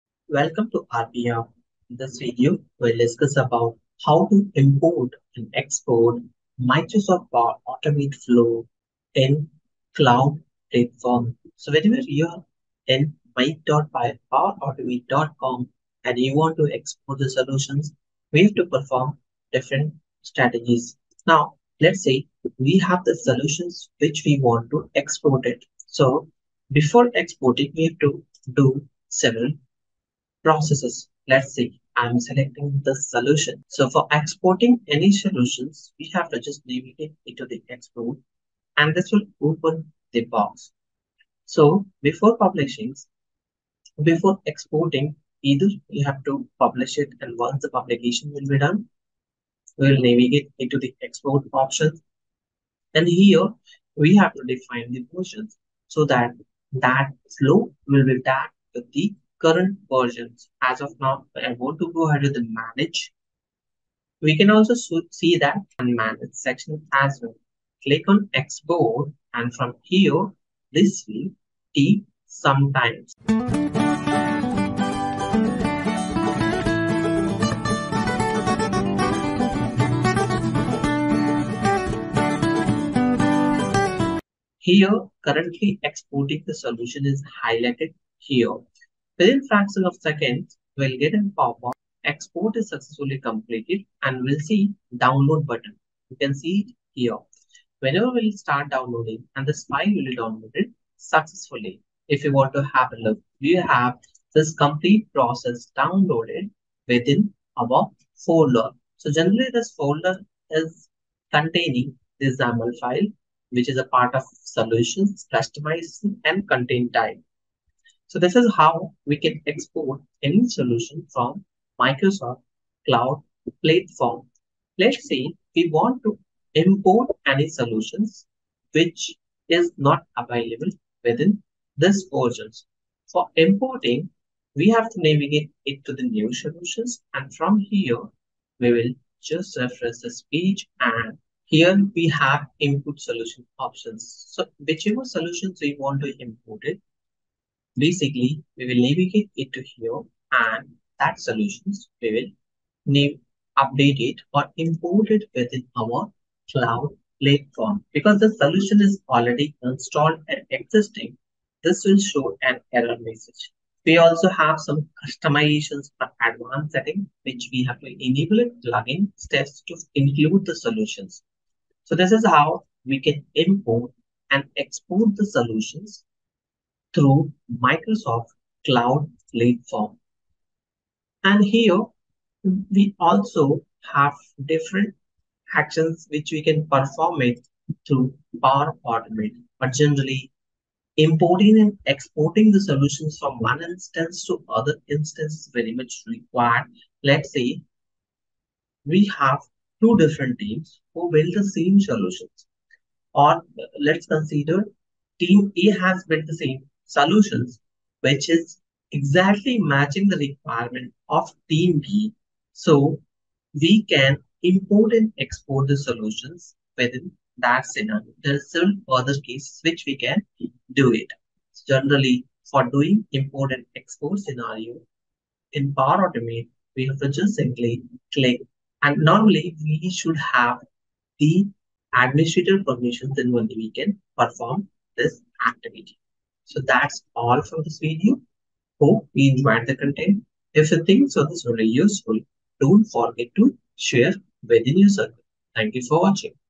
How to Import and Export Power Automate Cloud Flow
In this video, we will explain to you how to Import and Export Power Automate Cloud Flow.